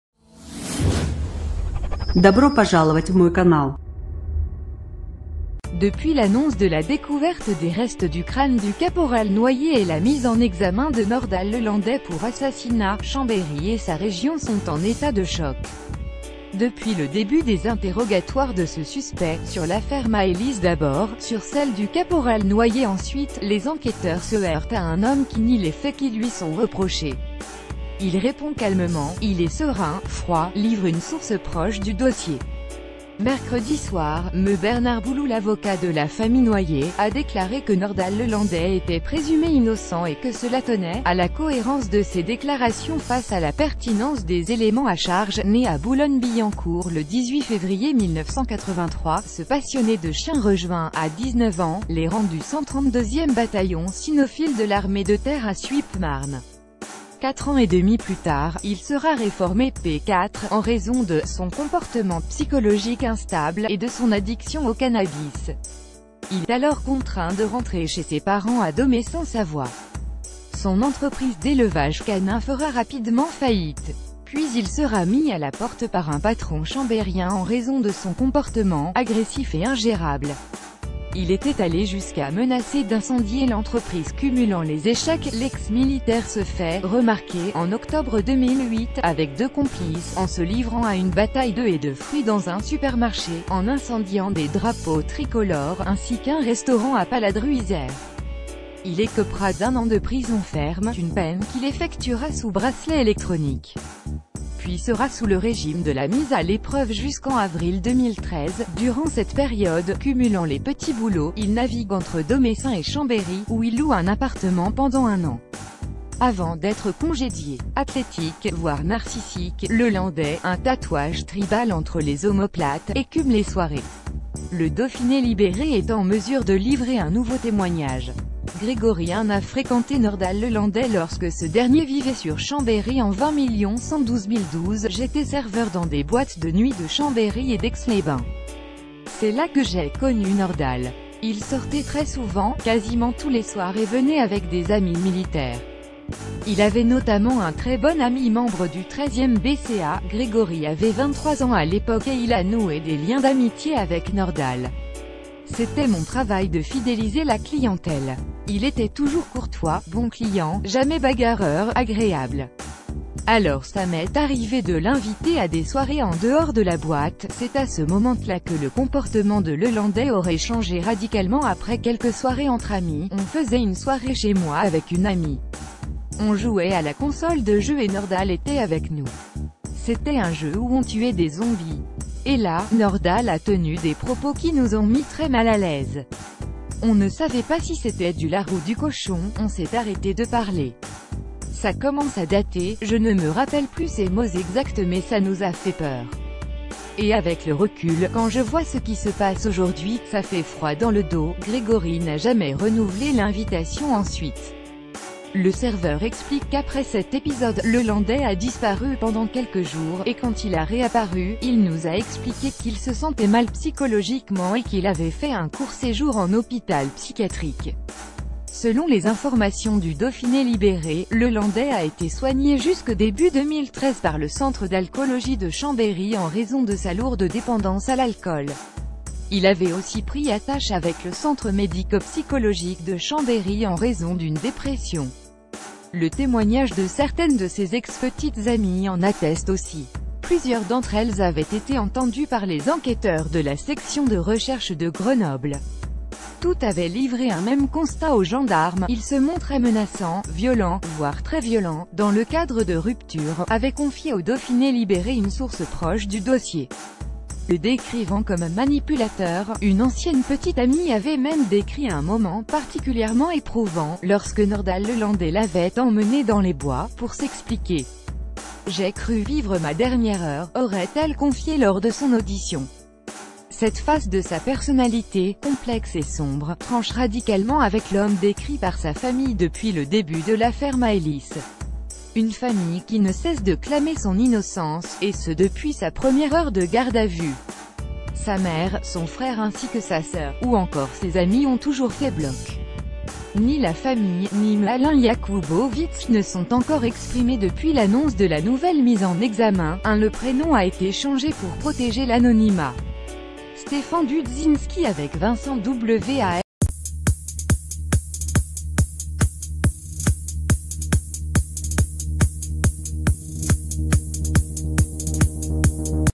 Nordahl lelandais, double visage , inquiétante personnalité
Depuis l’annonce de la découverte des restes du crâne du caporal Noyer et la mise en examen de Nordahl Lelandais pour assassinat, Chambéry et sa région sont en état de choc. Depuis...